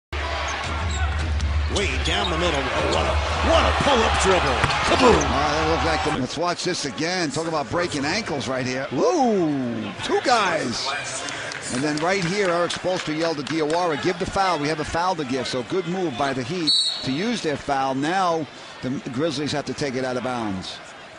NBA rank #1 anklebreaker Dwyane Wade
Dwyane Wade of Miami Heat did an insane ankle breaker on O.J. Mayo of Memphis Grizzlies.